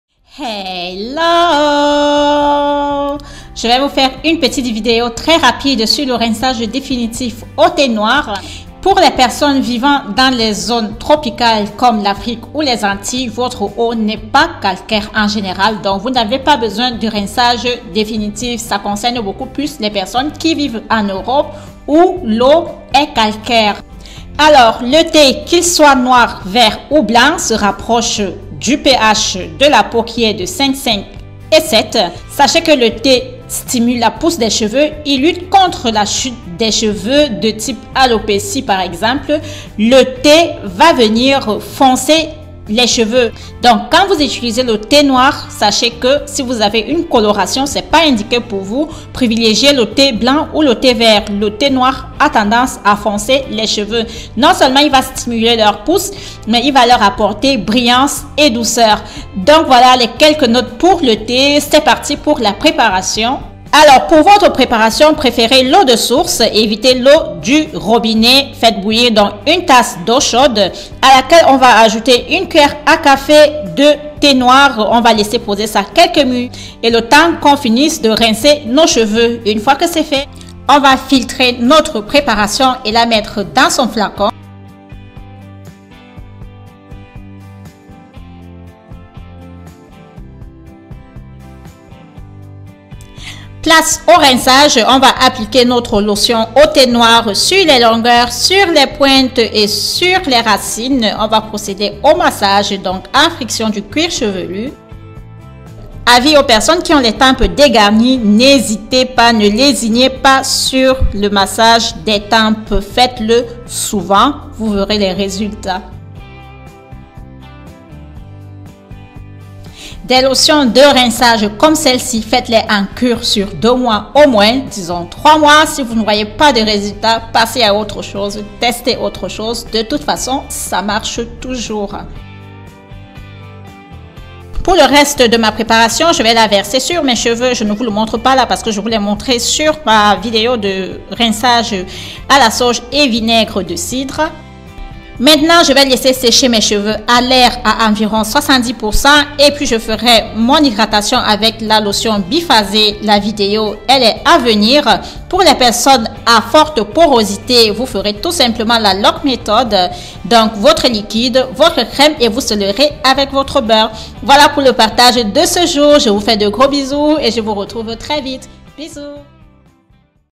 Rinçage au thé noir: Pousse et Brillance des cheveux
Hellooo!!!
Voici une proposition rinçage définitif pour la pousse, la brillance et la beauté des cheveux...